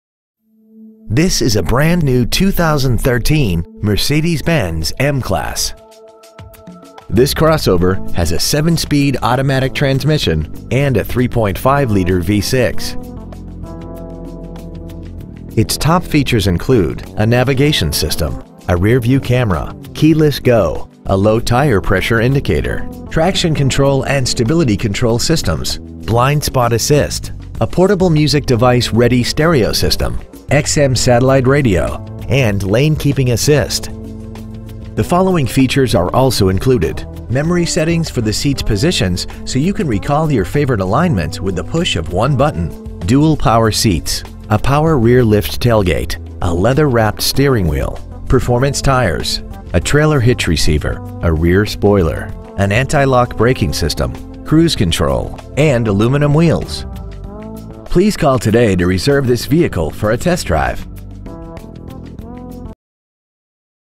2013 Mercedes-Benz ML350 Houston TX 77027
We are proud to present this 2013 Mercedes-Benz ML350 .

 Year : 2013
 Make : Mercedes-Benz
 Model : ML350
 Engine : Gas V6 3.5L/213
 Trans . : Automatic
 Exterior : Palladium Silver Metallic
 Miles : 5
 Interior : Gray